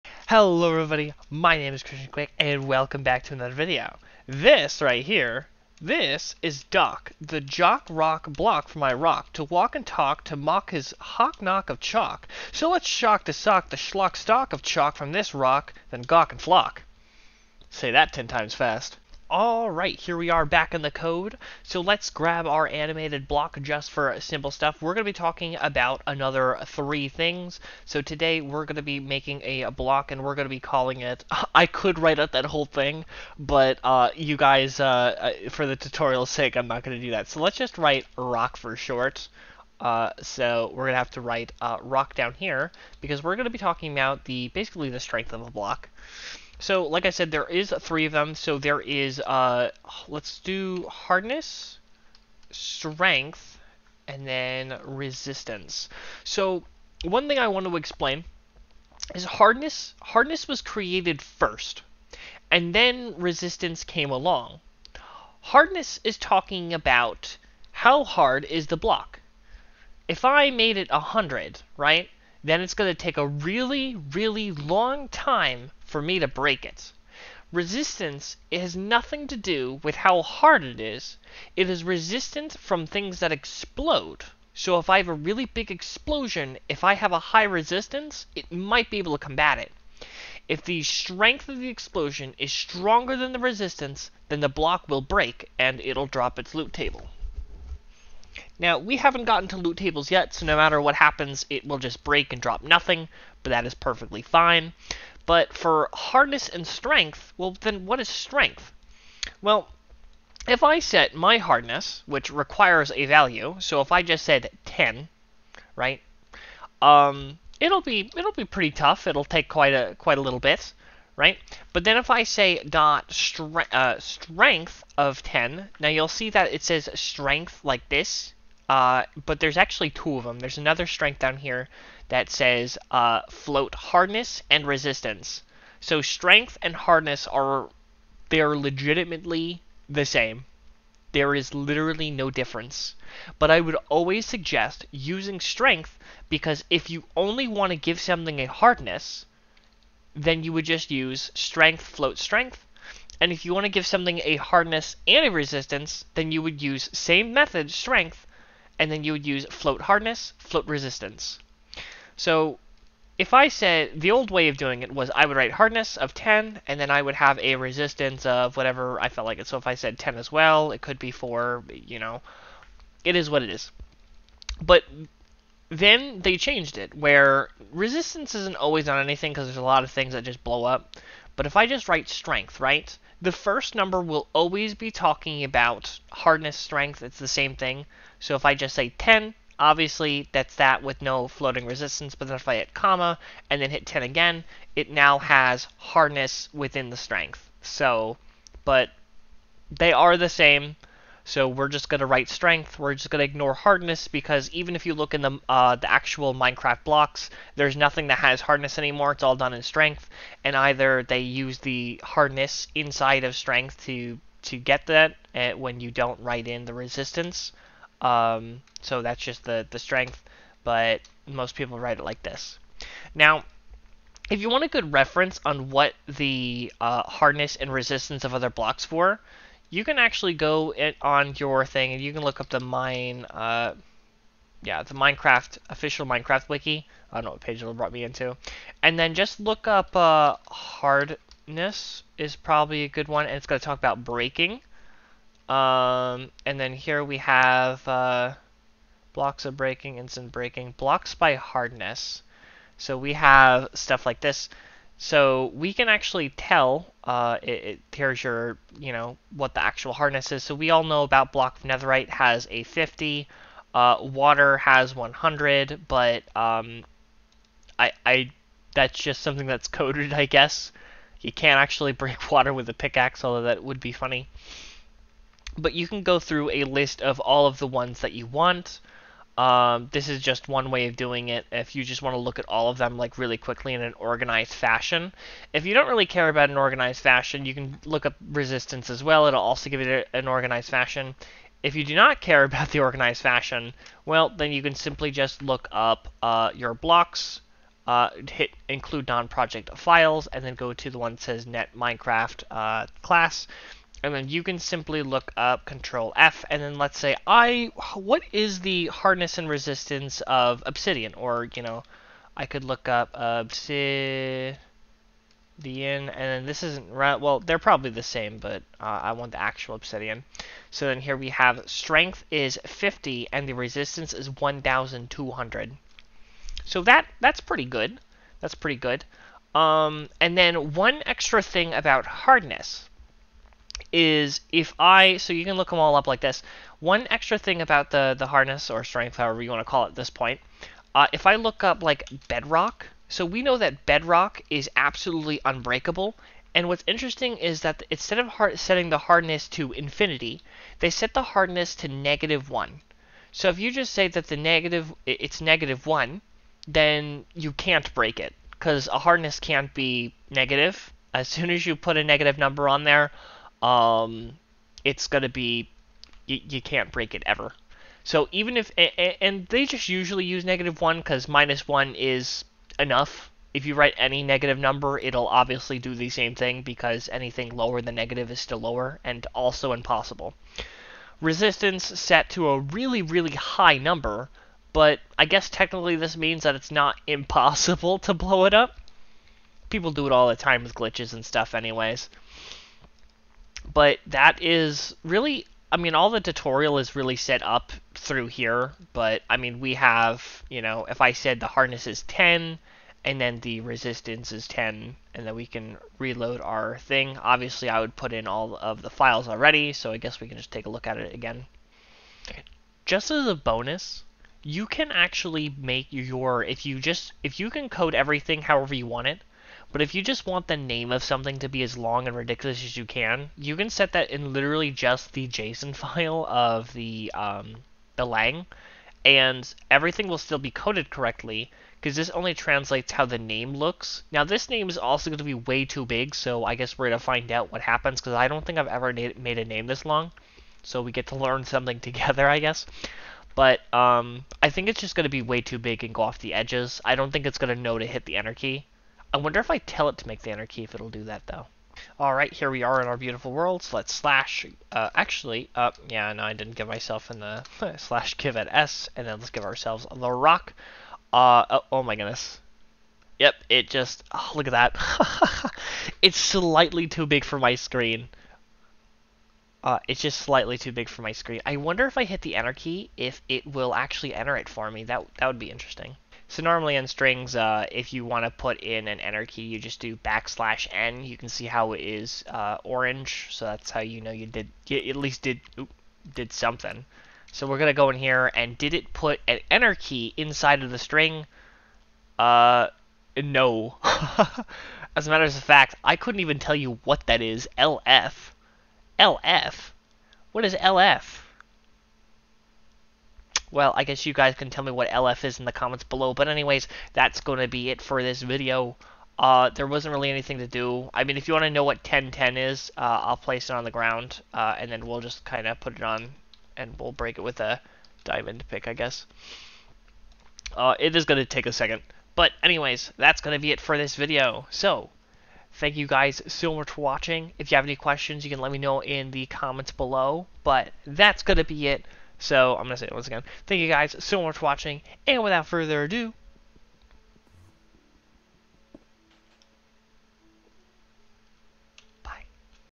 How To Use .strength.hardness & .resistance In Block Settings | Modding Tutorial #14
This video is coded in Fabric for Minecraft 1.20.1